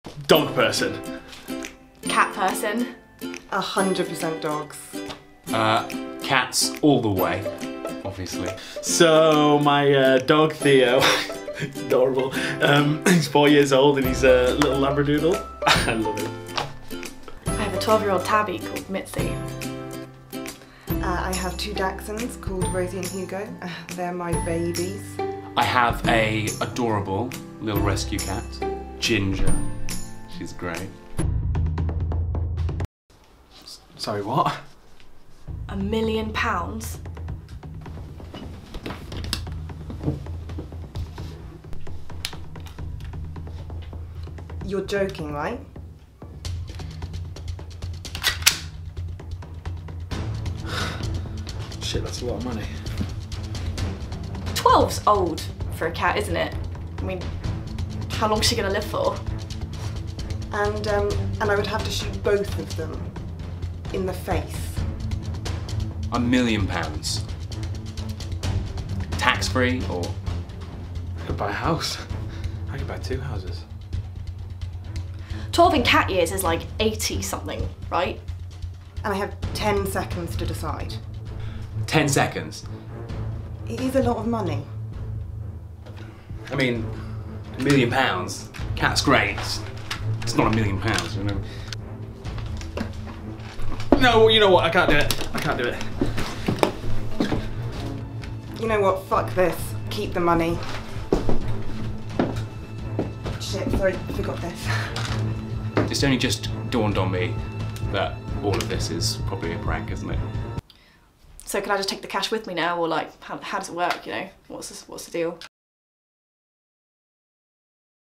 Dog People vs Cat People
Starring Christy Coysh, Kat Sadler, Billie Cooper and Jon Long
Written by Billie Cooper
Directed by Rob Hatton
Produced by Cameron Loxdale